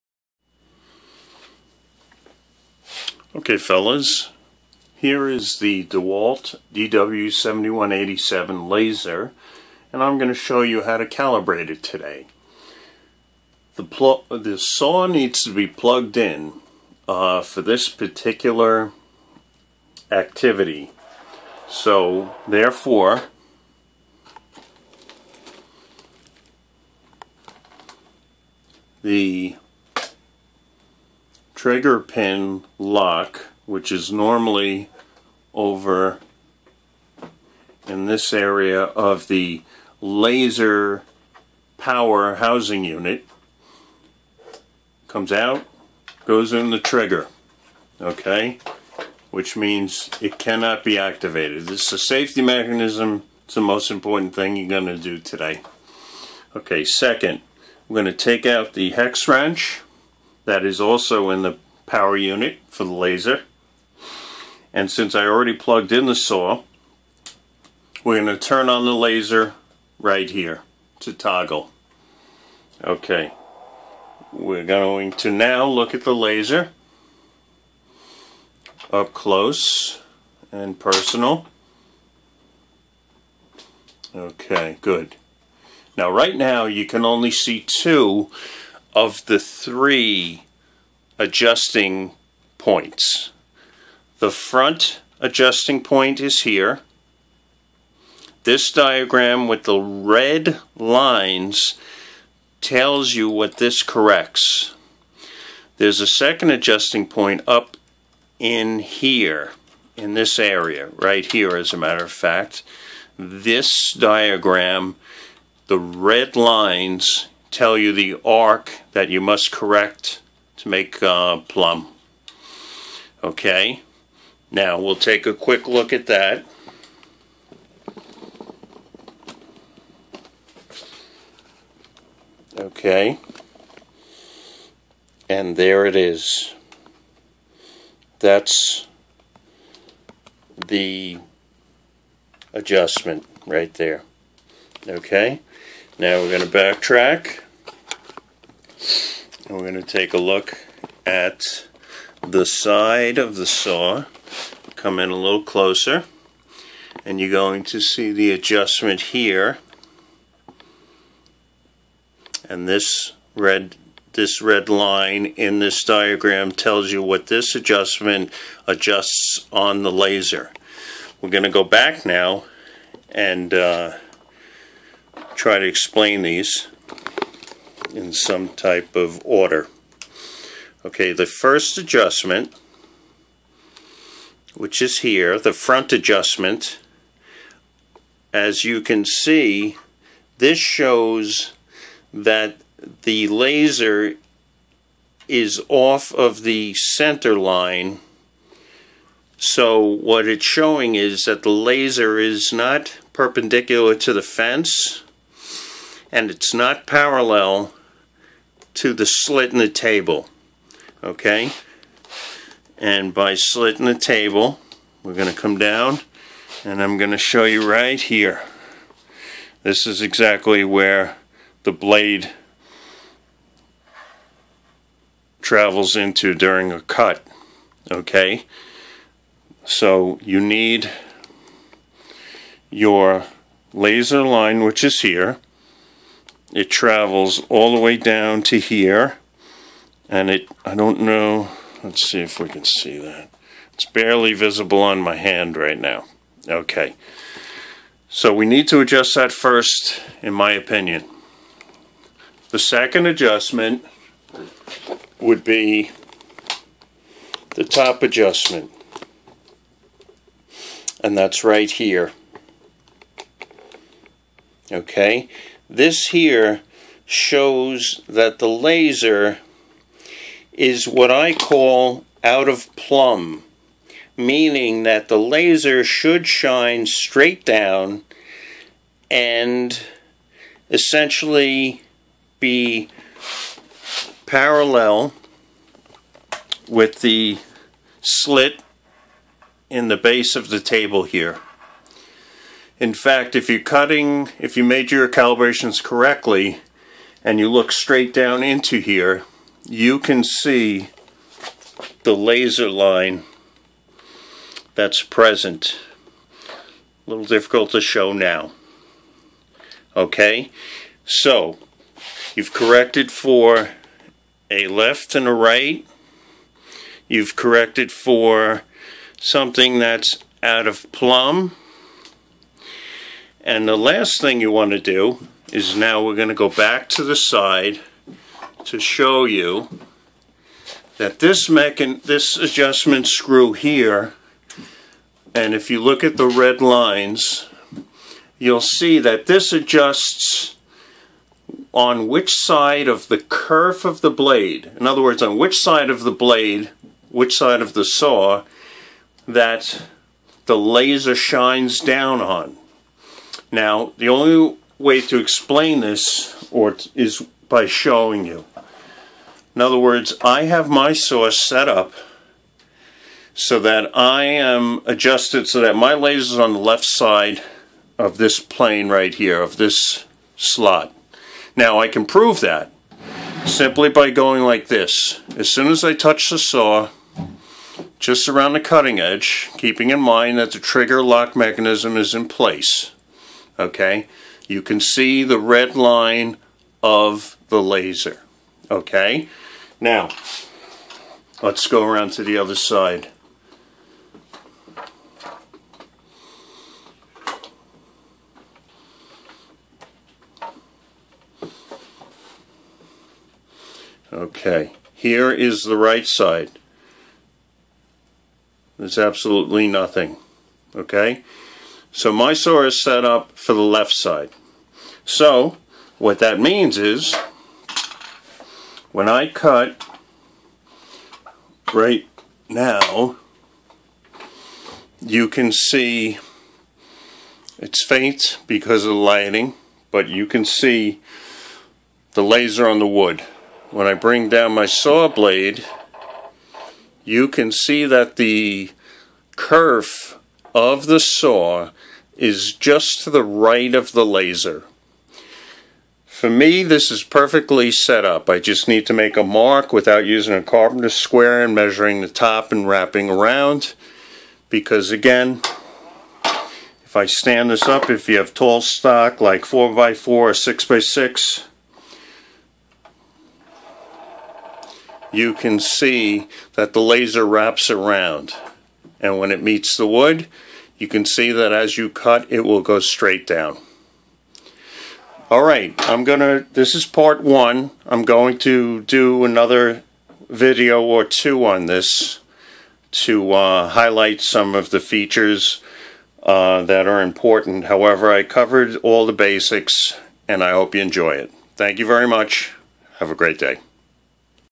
Laser Calibration for DeWalt Miter Saw Laser DW7187 Pt. 1
The DeWalt DW7187 Miter Saw Laser System adjustment/calibration procedure.